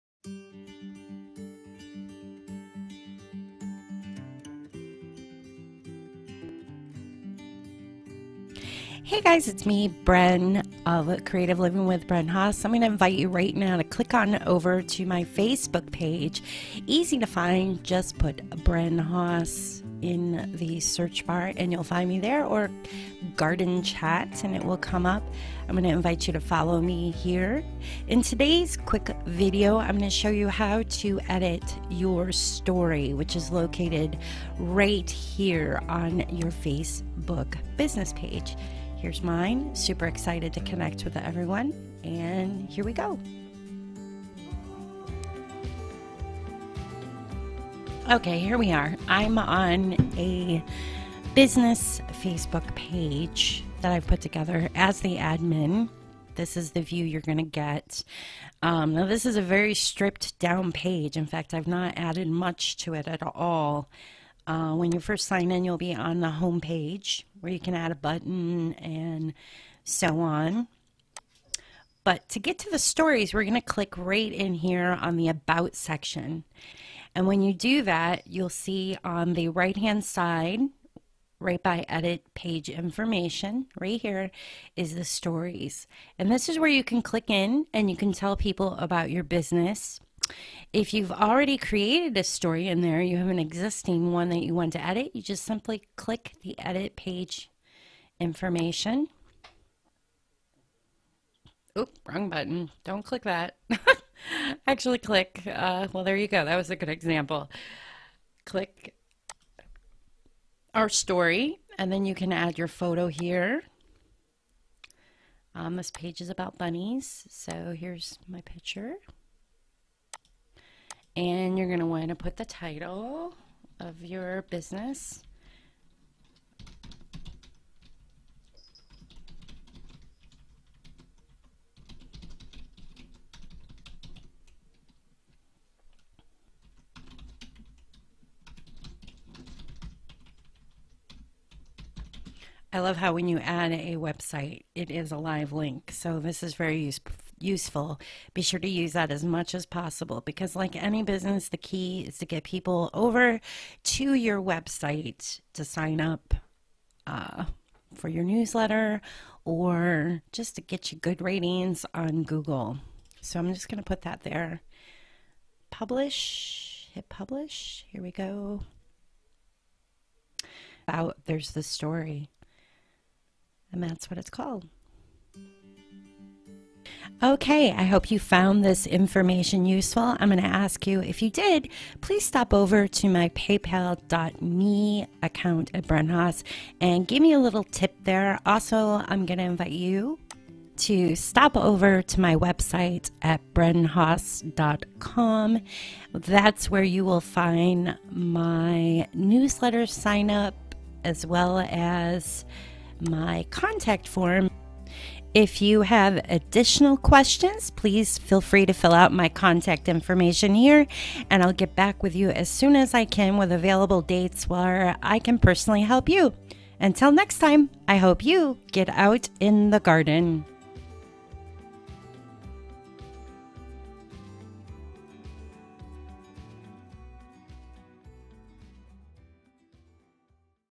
How to ADD A Facebook STORY to your Business Page on Computer
It is so important to have your Facebook business page filled out correctly.  In this post, I share a quick how to edit the 'story'.

This video is an update from last Facebook 'Story' how-to.  Social media is constantly changing so I will do updates as time allows.

If you would like to have me personally consult you about social media stop by my website contact page and fill out the contact form